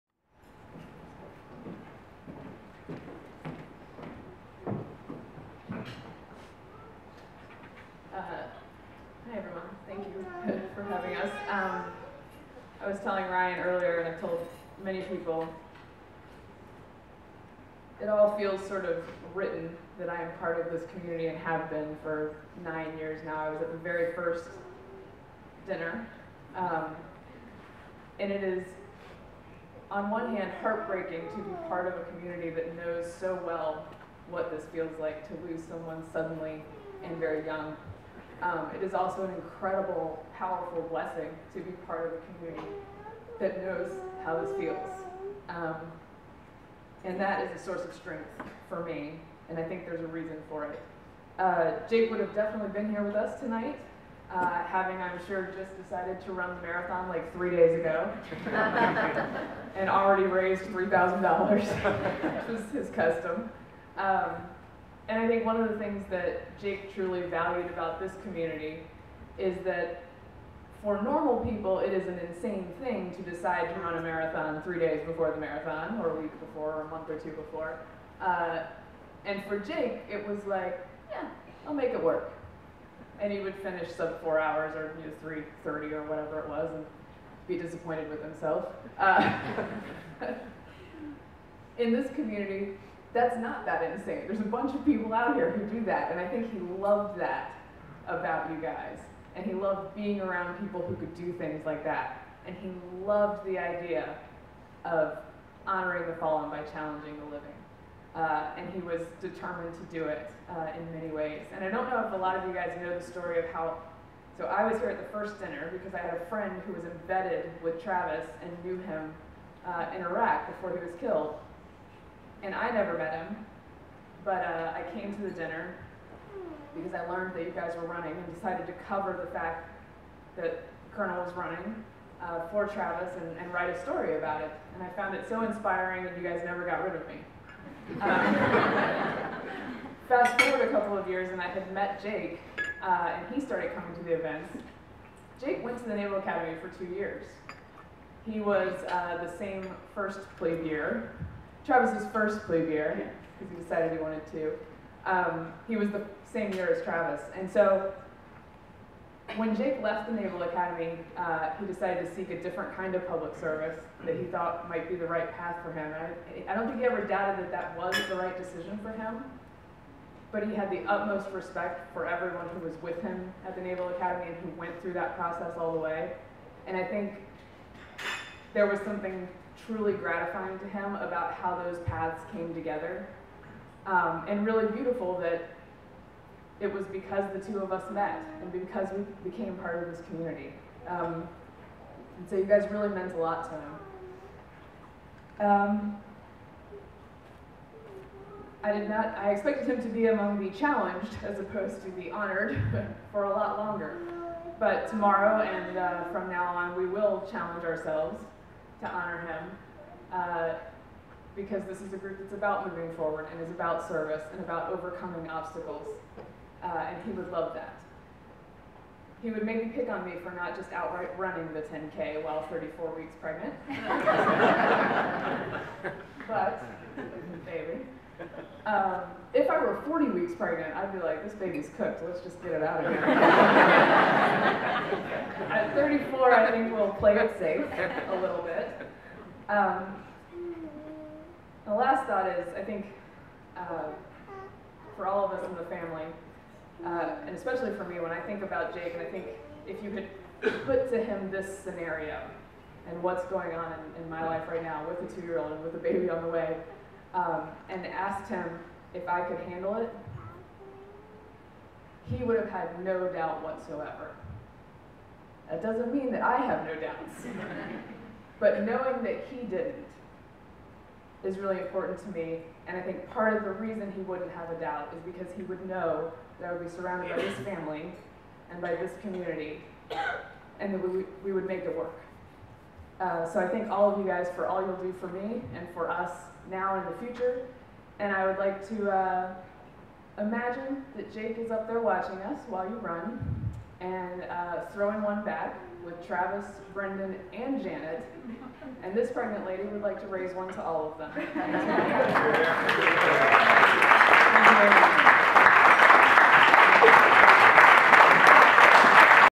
Mary Katharine Ham 2015 MCM Team Travis and Brendan Dinner
At the 2015 Marine Corps Marathon Team Travis and Brendan Dinner, we honored a long time TMF supporter, the late Jake Brewer. His wife and TMF Board Member Mary Katharine Ham spoke about Jake, and her motivation to participate in the MCM 10K the next day, even while 8 months pregnant. She is a true inspiration and the epitome of "If Not Me, Then Who..."